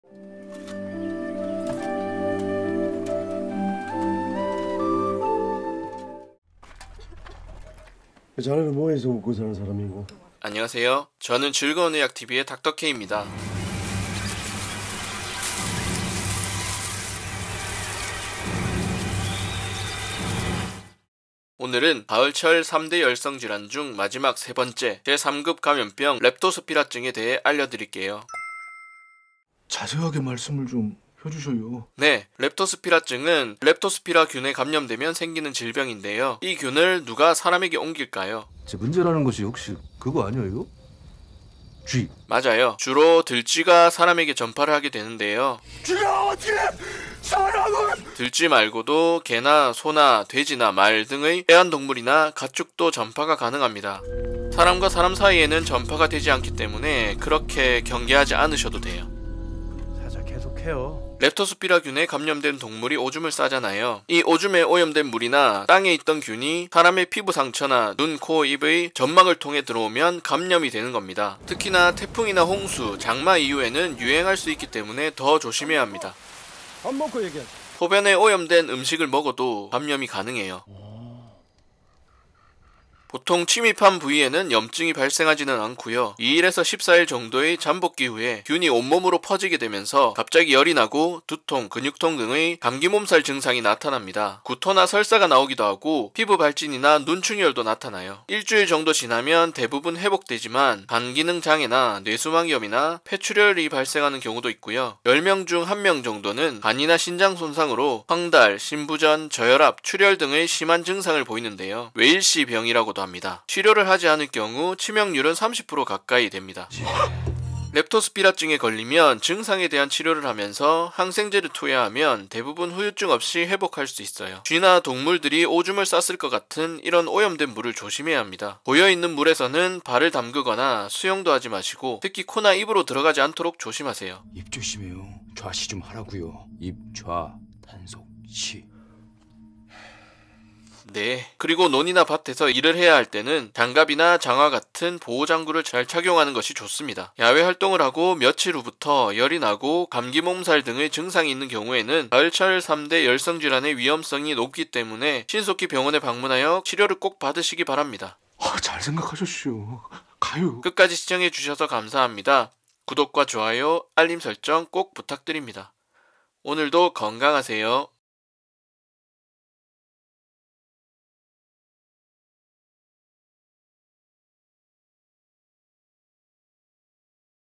렙토스피라증이 무서운 이유. Leptospirosis [feat. 손님]
가을철 3대 열성질환 중 하나로, 쯔쯔가무시증과 신증후군출혈열과 비슷한 증상을 보이는 질병.
렙토스피라증(Leptospirosis)에 대해 알아봅시다.

00:00 인트로
00:33 렙토스피라증의 원인과 감염경로
01:23 임상증상
01:58 치료 및 예방하는 방법